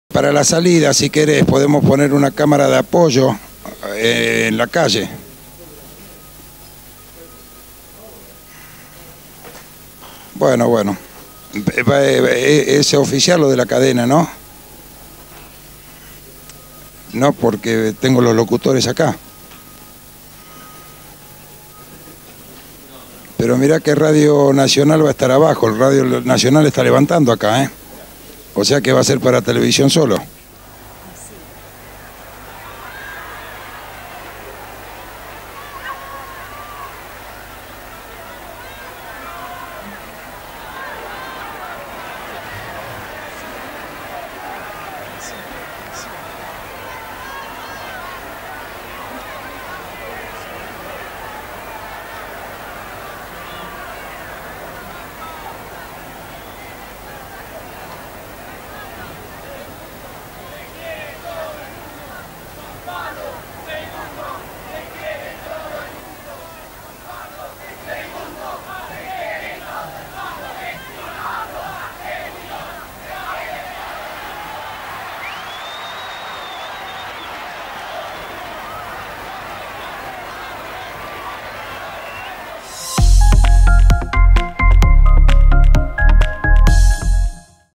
Día 72: El papa en la Rosada
12 de junio de 1982. Segundo día del papa en Argentina. Un periodista en off coordina detalles de la inminente cadena nacional, mientras Juan Pablo II está reunido con el presidente de facto Leopoldo Galtieri y los comandantes en jefe de Marina y Fuerza Aérea.